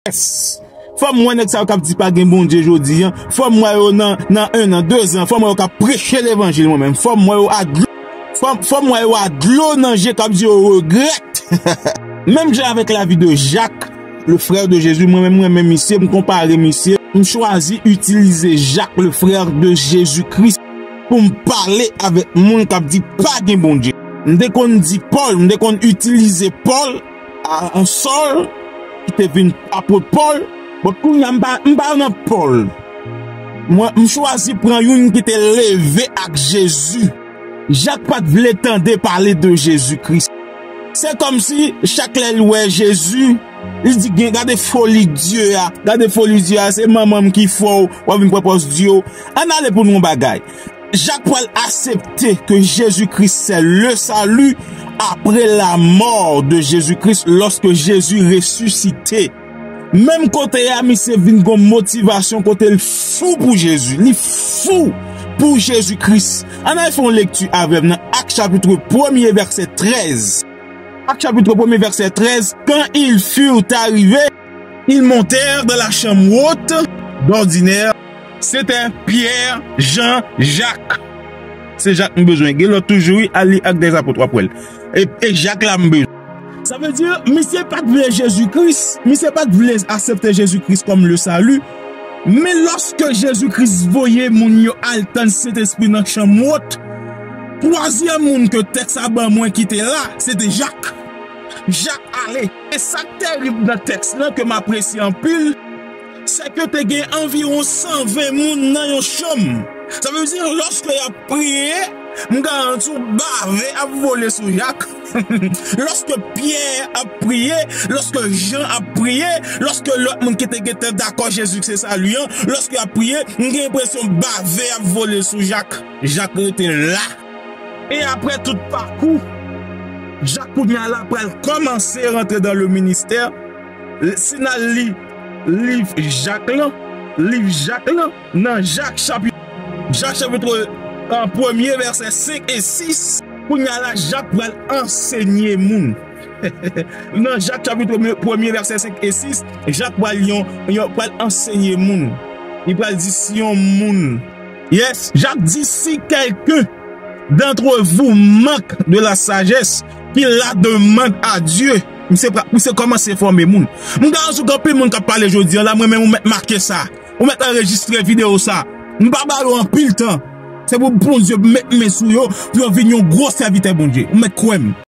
Apôtre Paul avek Jacques frère de Jésus kache anndan chak nèg ki ap di pa gen Bon Dieu yo.
Jou Pou n dekouvri sa gentan planifye Ann swiv

kontinye abone guys just pou motive m plus merci